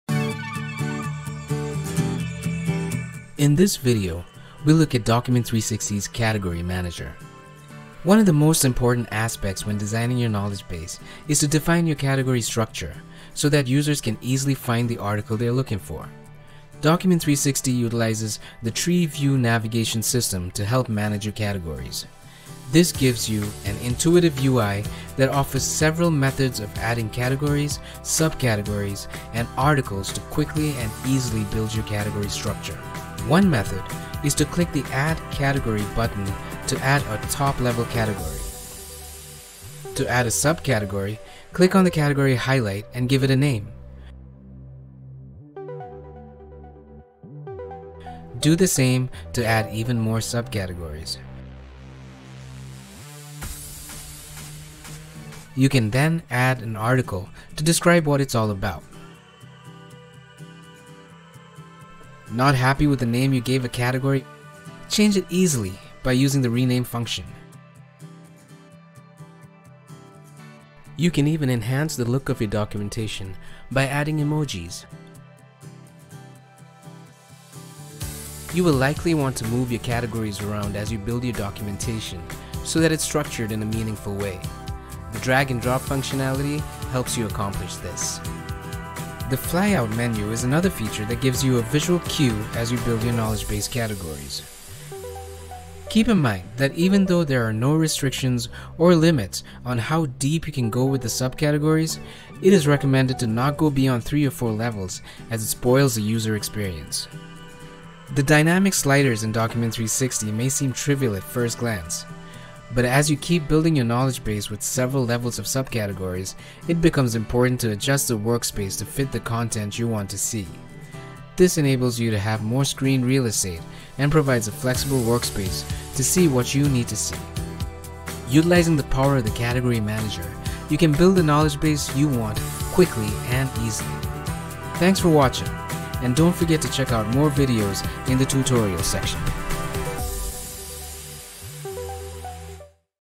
How to Maintain your Document Effectively with Category Manager
In Document360, Categories are used for organizing and/or display of certain buckets of the knowledge base. It works like a folder that contains a group of related content. Categories and sub-categories provide a clean structure for the documentation.

Based on your requirement, you can organize your documentation into a category hierarchy structure — which can go up to six-levels

▸Category
▸Sub-Category
▸Article